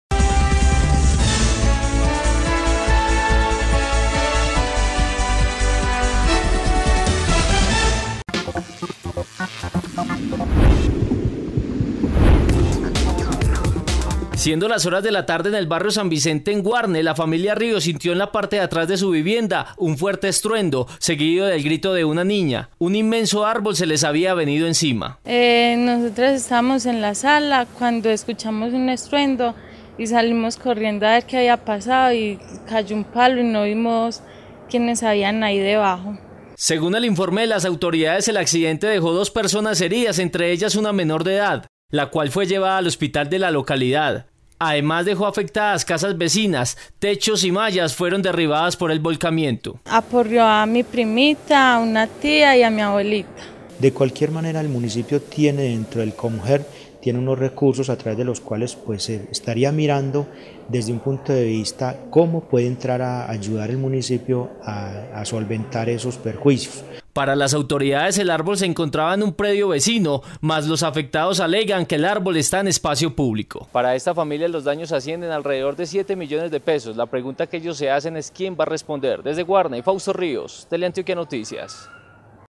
En Guarne un árbol cayó sobre una casa y aún no se sabe quién responde por los daños
Una familia en Guarne está desesperada porque le cayó un árbol sobre su vivienda, dejándole daños cercanos a los 7 millones de pesos. El problema radica en que no saben quién les responderá por la situación, pues unos dicen que el árbol estaba en espacio público y otros que era propiedad de un vecino.